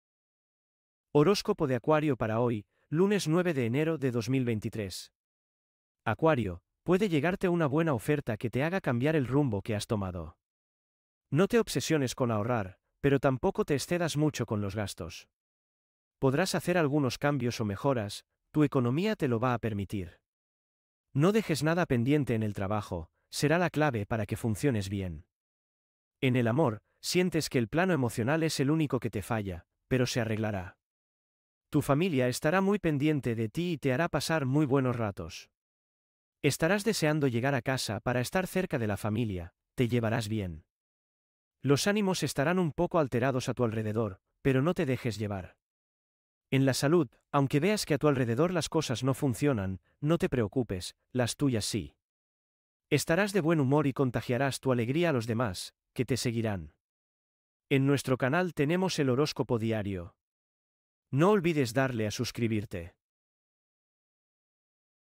HORÓSCOPO DIARIO 💘🌞🔥 ACUARIO ♒️ LUNES, 9 DE ENERO DE 2023
Horóscopo de Acuario para hoy, lunes 9 de Enero de 2023. El horóscopo para hoy: descubre tu futuro gracias a mi horóscopo diario para ti.